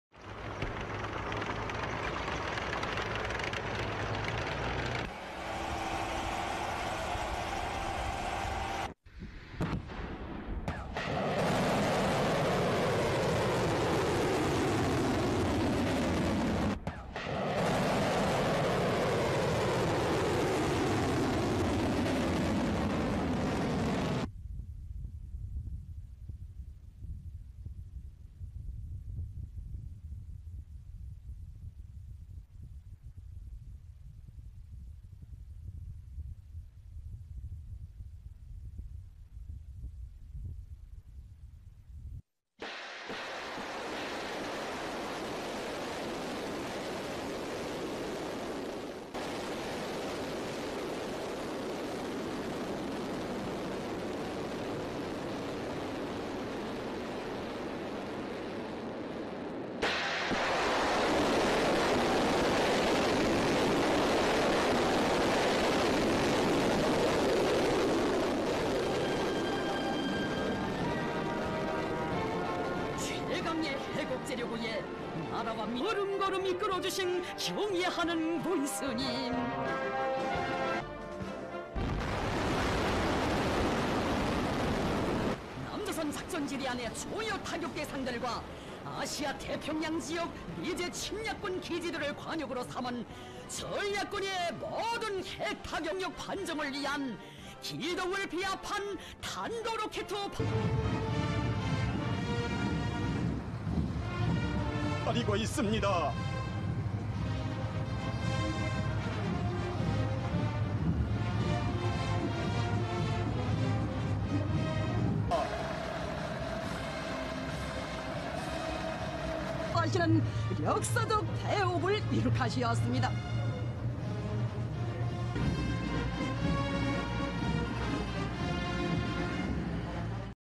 North Korea tests multiple land-to-ship missiles off its east coast - Daily Mail
North Korea fired what appeared to be multiple land-to-ship missiles off its east coast on Thursday morning, South Korea's military said. In total four cruise missiles (one pictured left, and a launcher seen right) were fired from the North Korean coastal city of Wonsan, Gangwon Province, a US defense official told CNN. The missiles flew around 124 miles, South Korean officials said. Wonsan is 113 miles from the South Korean capital of Seoul.